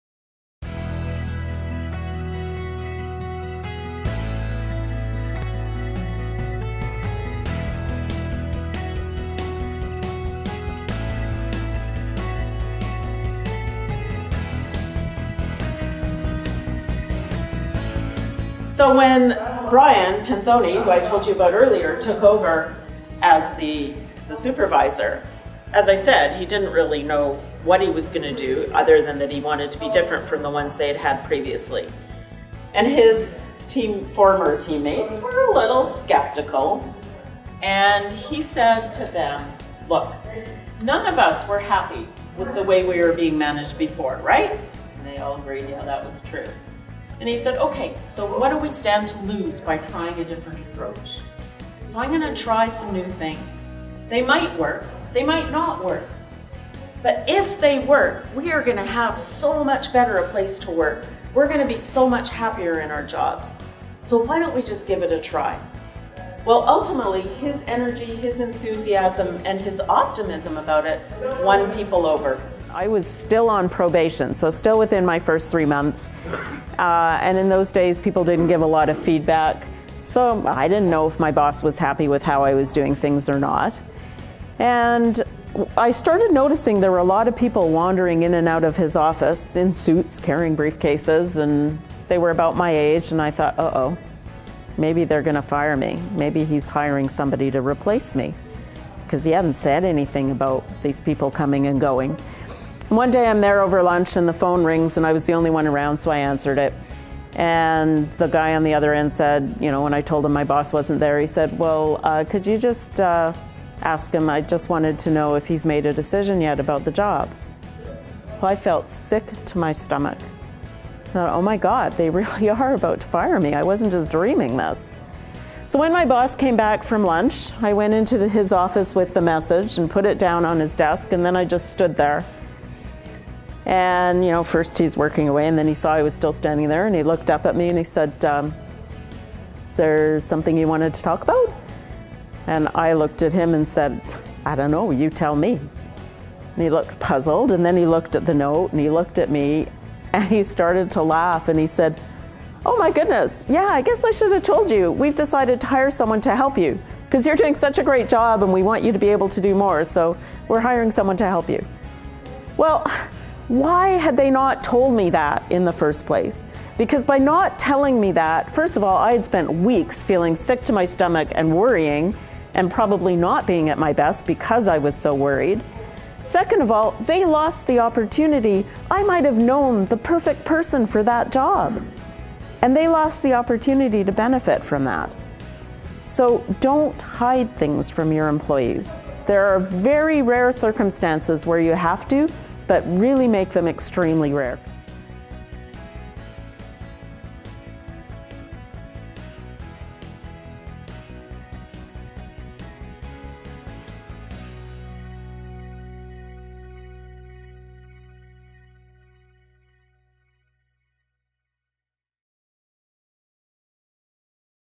People - 2nd P in the 3P Profit Formula
People -- inside & outside your organization -- are key to success.  Relationships with (and among) staff, customers, prospects, suppliers, distributors, lenders, the public, etc. are all crucial in ensuring great customer experiences. Tema Frank gives examples in this clip from a talk about Supervising for Success.